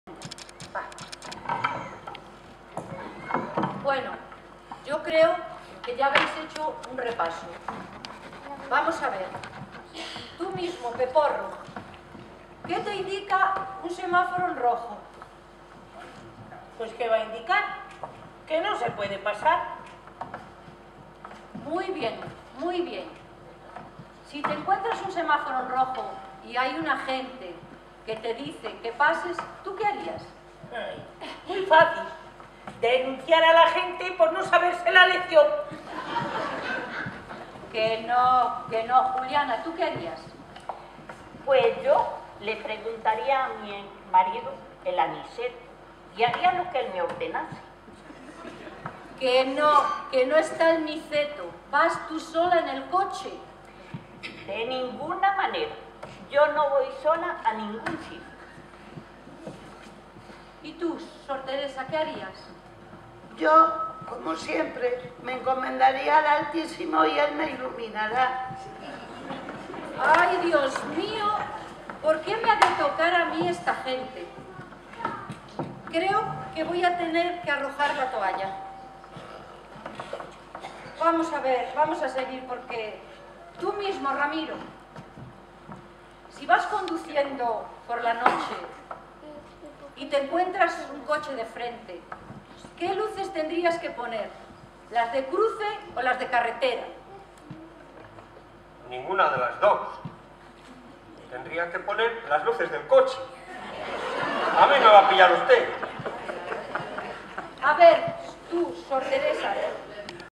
Festival Solidario 3 Cacabelos
Tercera parte del Festival Solidario de Voluntarios Flavium de Cacabelos el 27 de octubre de 2018